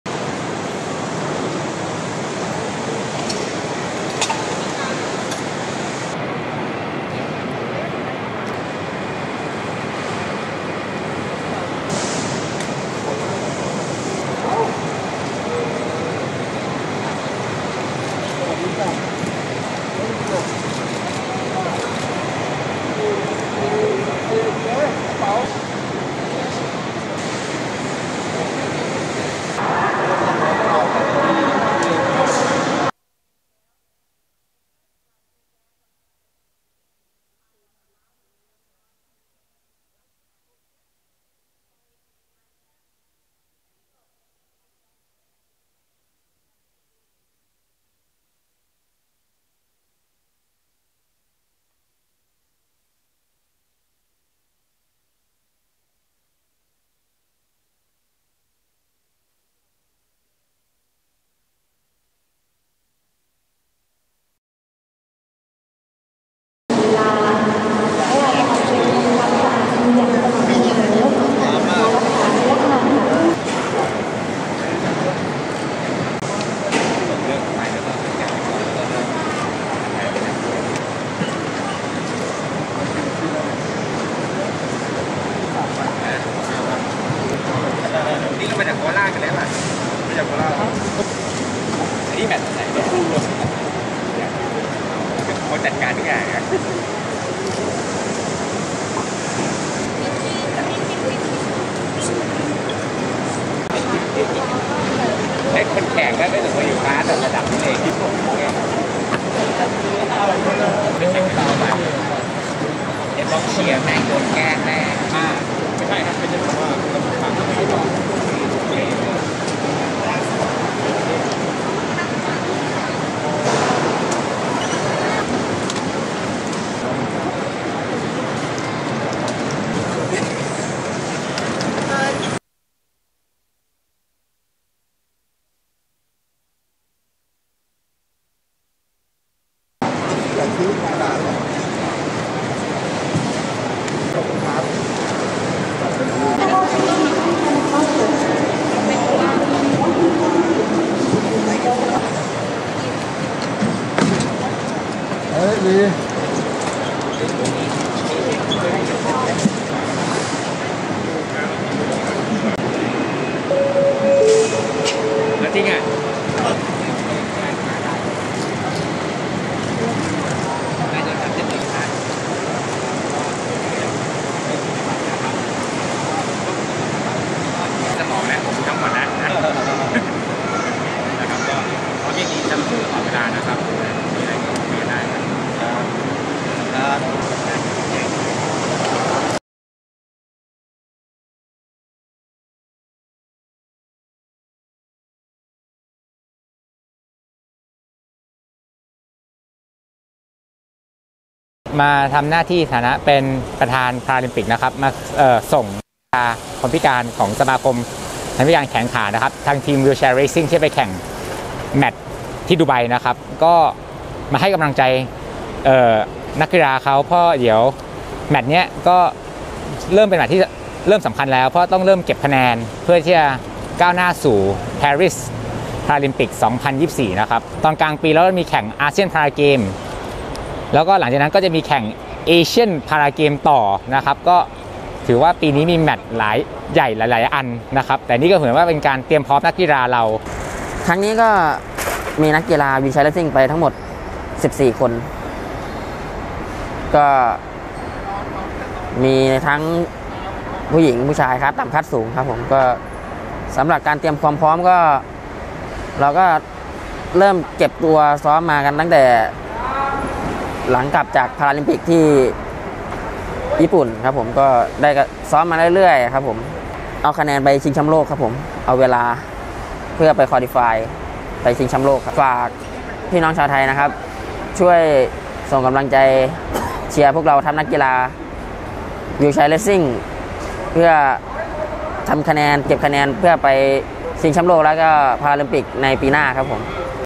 ภาพข่าวคุณต่อยส่งนักกีฬาวีลแชร์เรชชิ่งเพื่อแแข่งขันSharjah International Open Para Athletics 2023
1.คุณณัยณพ ภิรมย์ภักดี
ประธานพาราลิมปิกไทย
2,พงศกร แปยอ
นักกีฬาวีลแขร์เรชชิ่ง